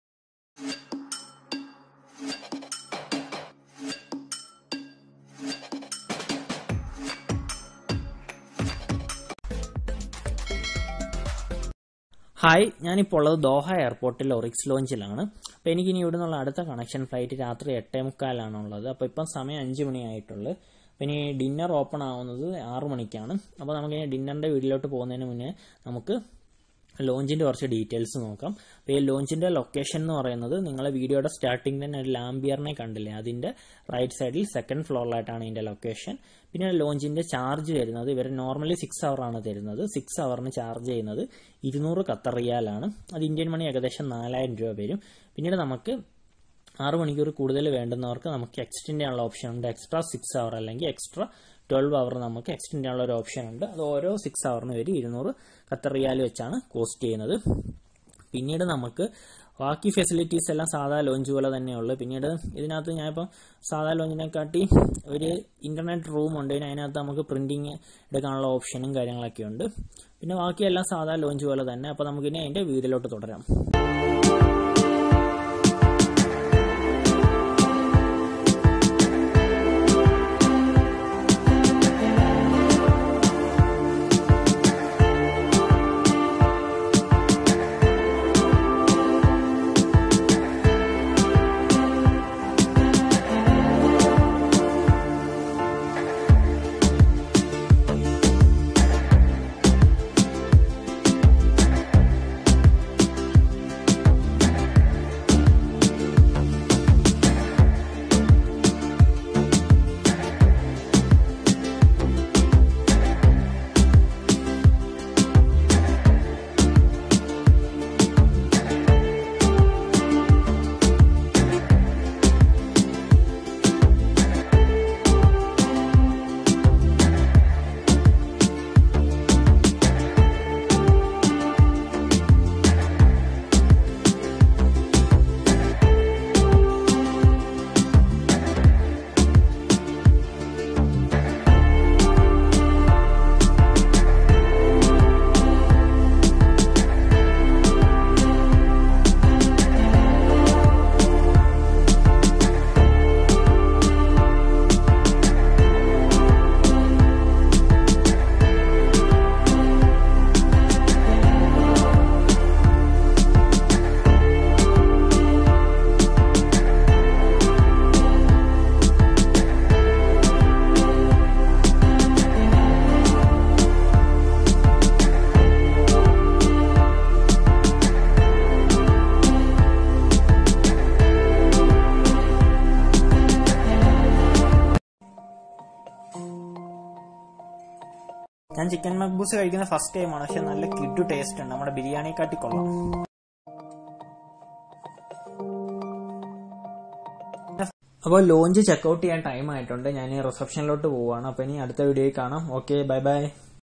Oryx Lounge | Best lounge in Doha international Airport / Malayalm review
Oryx Lounge Doha Hamad International Airport Qatar

Lounge Facilities:
# Hot and Cold Buffet.
# Locker Room.
# Washroom with Shower.
# Complimentary Wifi.
# Family Area.
# Quiet Room with recliners.